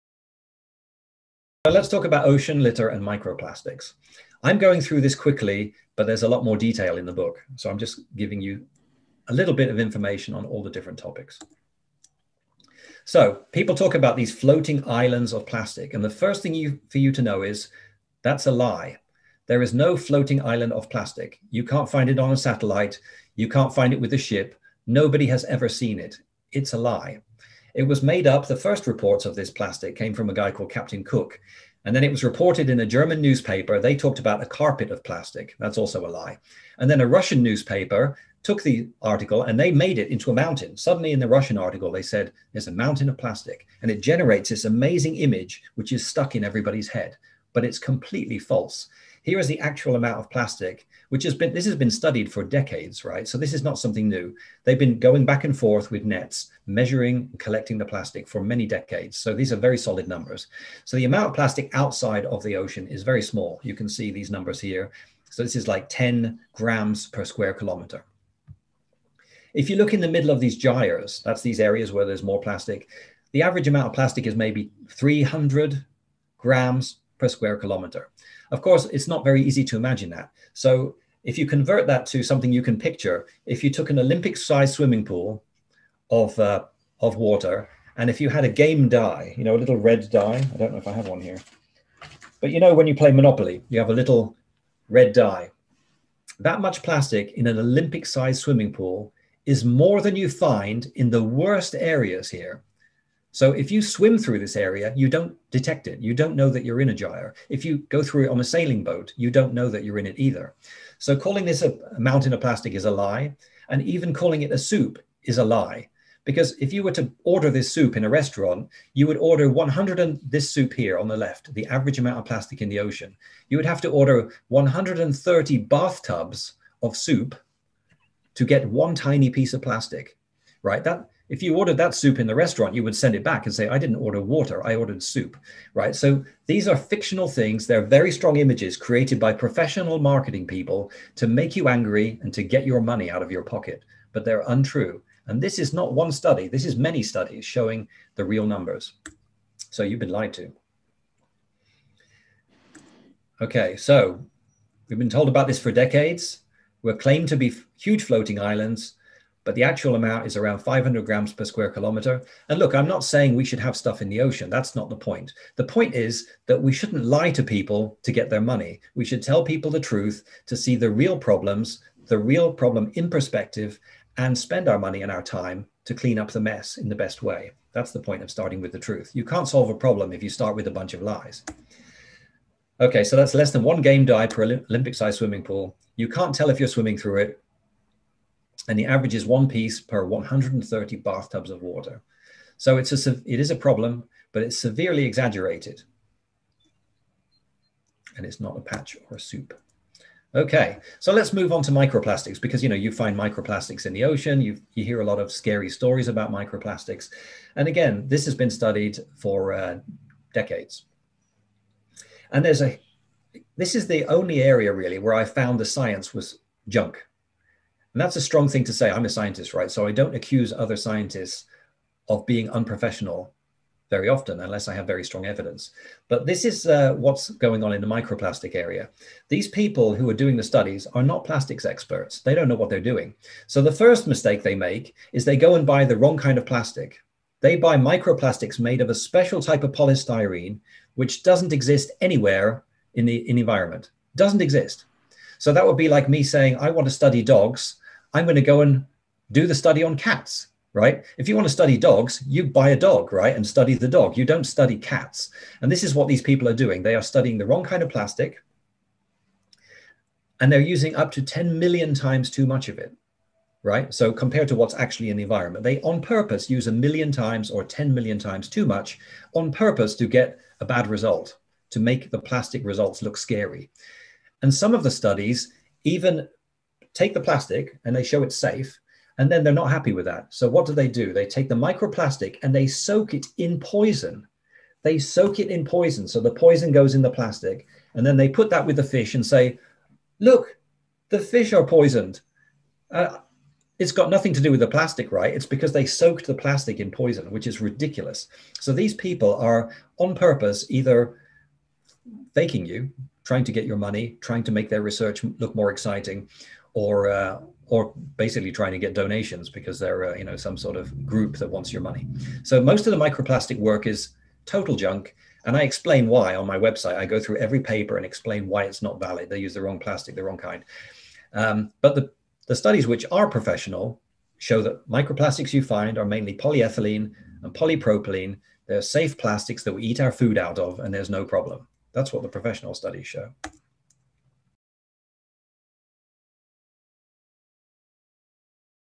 L' Isola che non c'è - Chris DeArmitt per RaccoltalaGiusta
Chris DeArmitt, scienziato e autore del libro The Plastics Paradox: Facts for a Brighter Future, racconta delle sue ricerche, basate su più di 400 articoli scientifici, nel webinar The Plastics Paradox.

Chris DeArmitt parla delle isole galleggianti e delle loro dimensioni attuali all’interno degli oceani: quanto sono grandi realmente questi accumuli di rifiuti? L’ammontare effettivo delle plastiche presenti nei mari è di 500g per km quadrato...

fonte: le tesi espresse da Chris DeArmitt sono fondate sugli studi analizzati e consultabili scaricando The Plastics Paradox